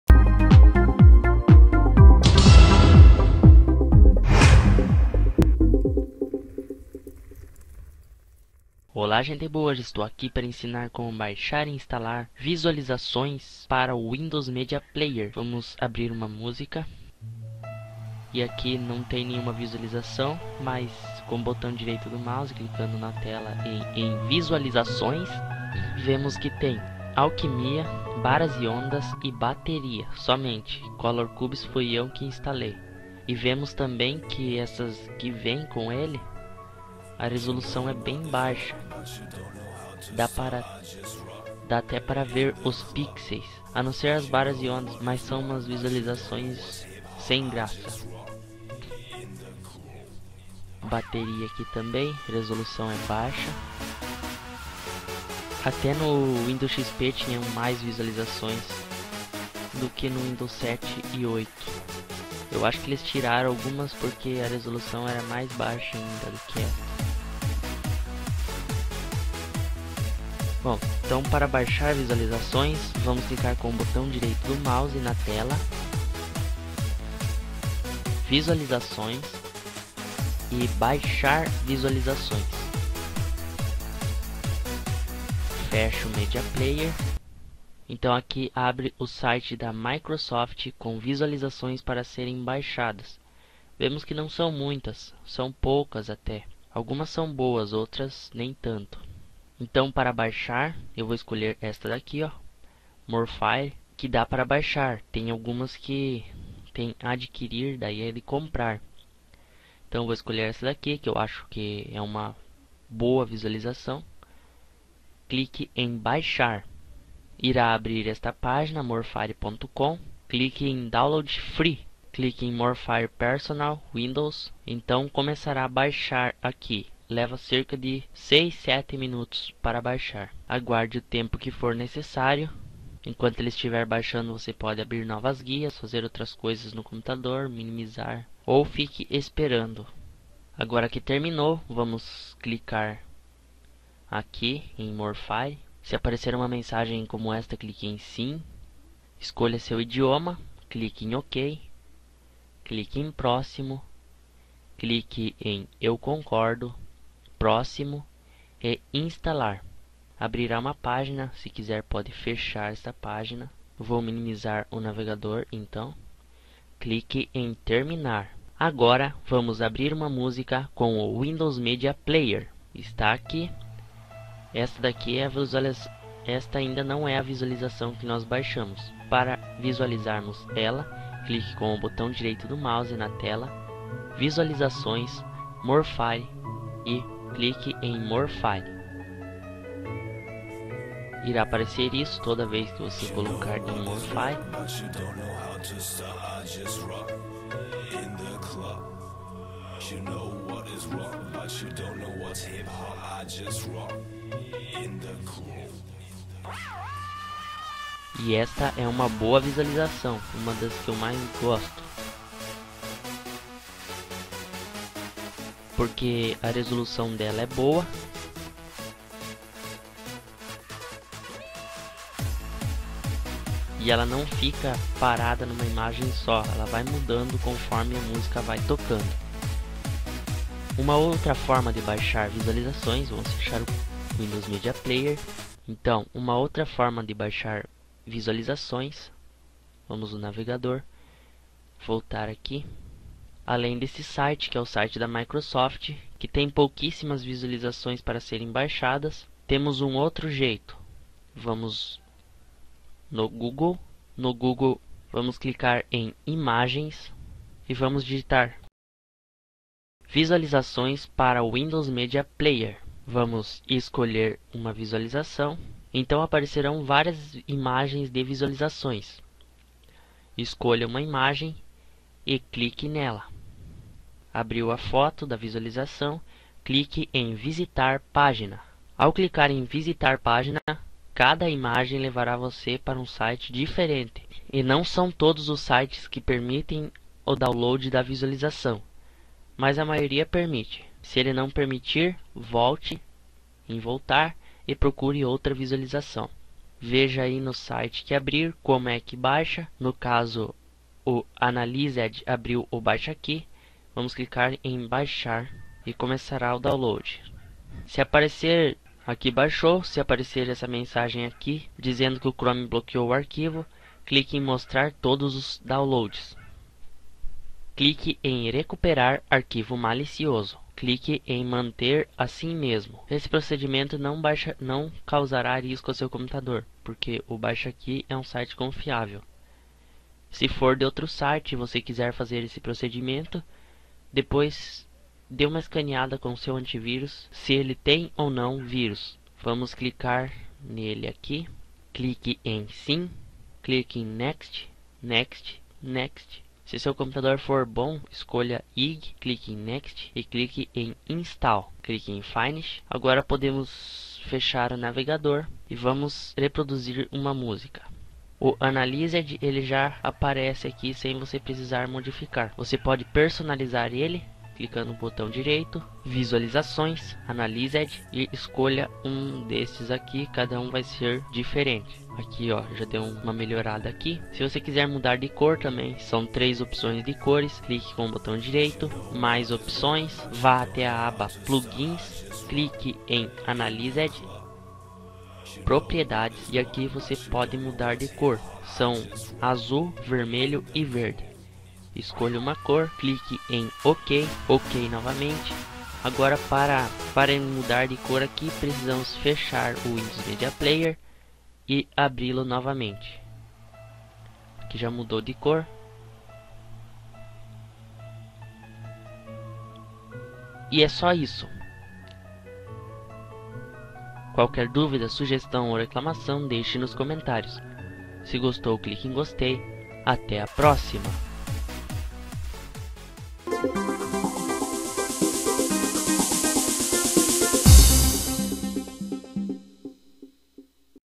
Como Baixar e Instalar Visualizações para o Windows Media Player
Este é mais um tutorial rápido de GL Produções.

Qualquer dúvida, sugestão ou reclamação, deixe nos comentários abaixo.

Se você gostou do vídeo e ele foi útil para você, clique em "GOSTEI" logo acima desta descrição.